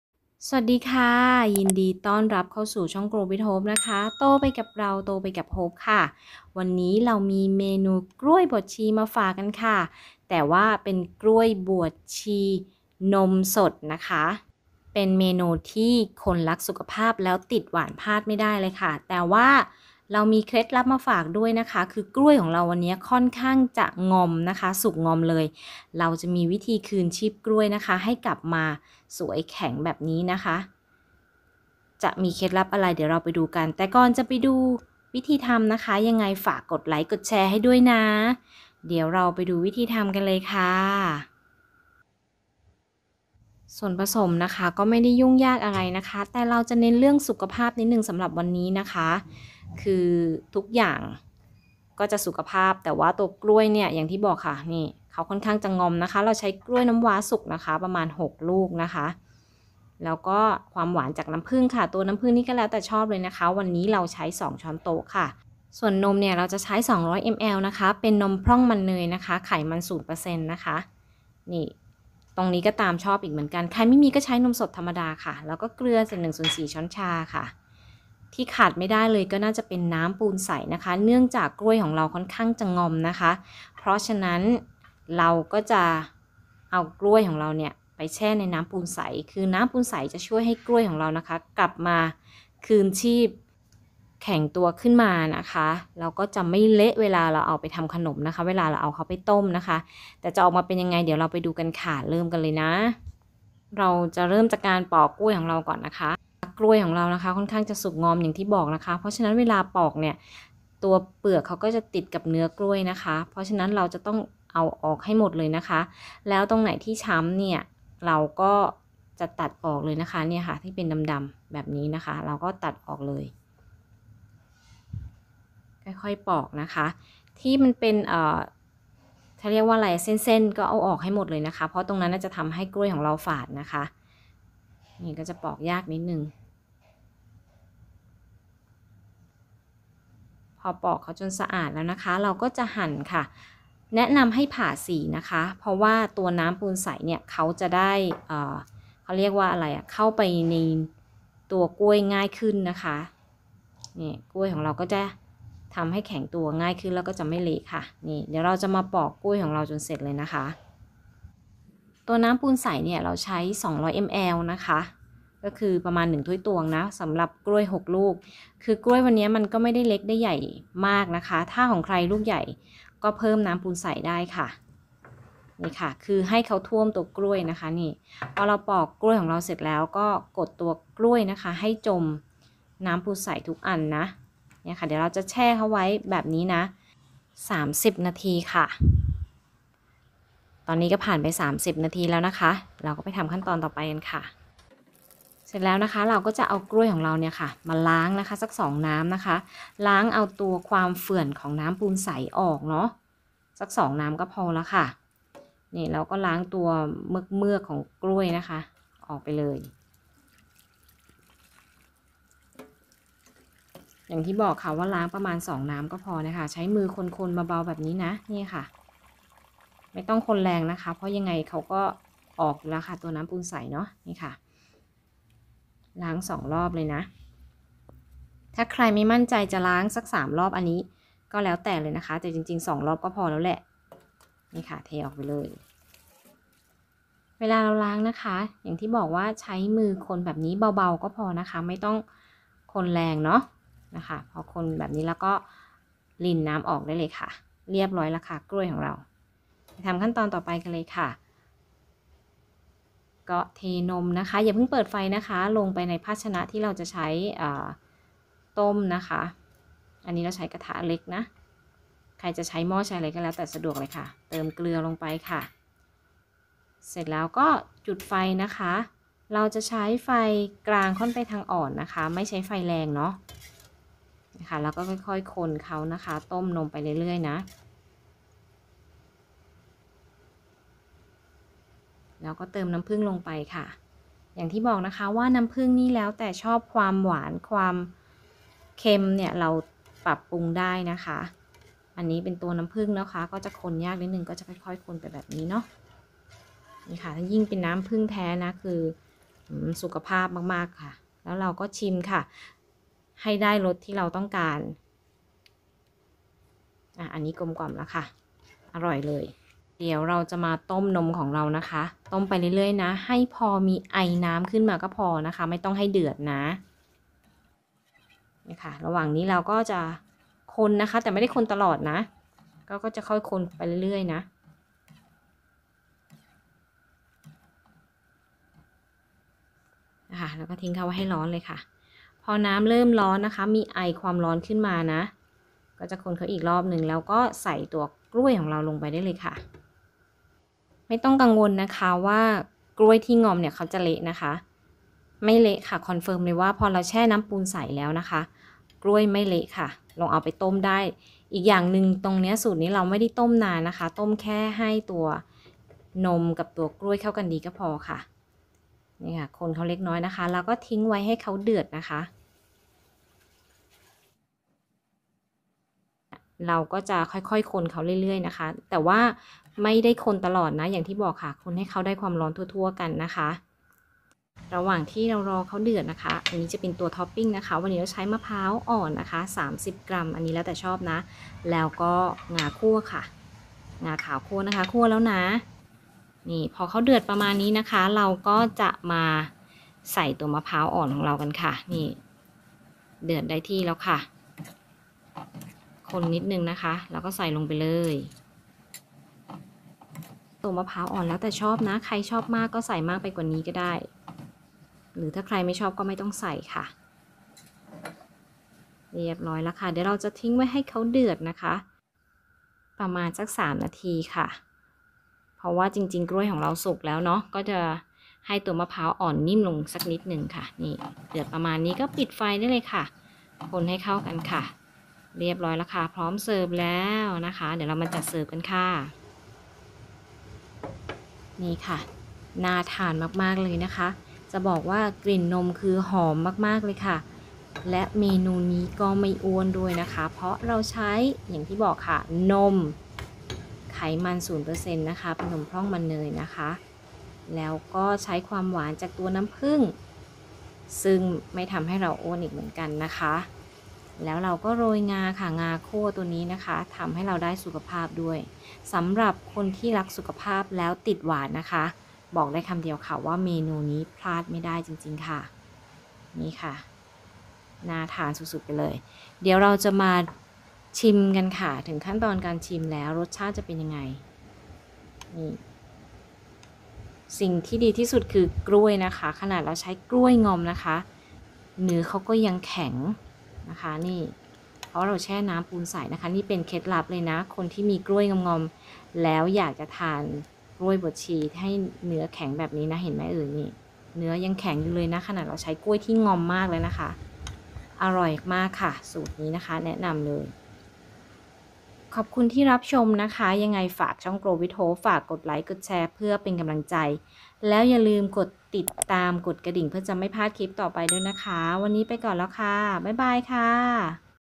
แบ่งปัน เคล็ดลับทำอย่างไรให้กล้วยสุกงอมกลับมาเนื้อแข็งหนึบและไม่เละ ด้วยเมนู กล้วยบวชชีนมสด
สวัสดีค่ะ วันนี้มาแบ่งปันสูตร กล้วยบวชชีนมสด เพื่อสุขภาพ มีเคล็ดลับทำอย่างไรให้กล้วยสุกงอมกลับมาเนื้อแข็งหนึบและไม่เละ หอม หวานด้วยธรรมชรติและได้สุขภาพ

กล้วยน้ำว้าสุกงอม 6 ลูก
นมสดพร่องมันเนย 200 ml.
น้ำปูนใส 200 ml.

Ingredient

 6 ripe bananas (Namva bananas)
 200 ml. skim milk
 2 tbsp of honey
 1/4 tsp salt
 200 ml of lime water.

สามารถติดต่อพูดคุยกับ Grow With Hope ได้ที่👉